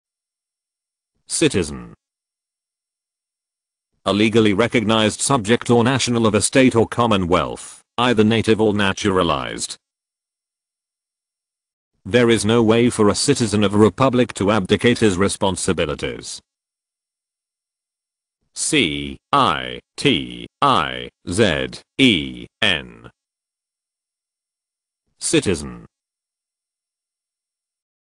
Citizen - Definition and How To Pronounce
Title: citizen

A legally recognized subject or national of a state or commonwealth, either native or naturalized.

There is no way for a citizen of a republic to abdicate his responsibilities.

How to pronounce citizen
definition of citizen
audio dictionary
How to say citizen
What is the meaning of citizen
Pronounce citizen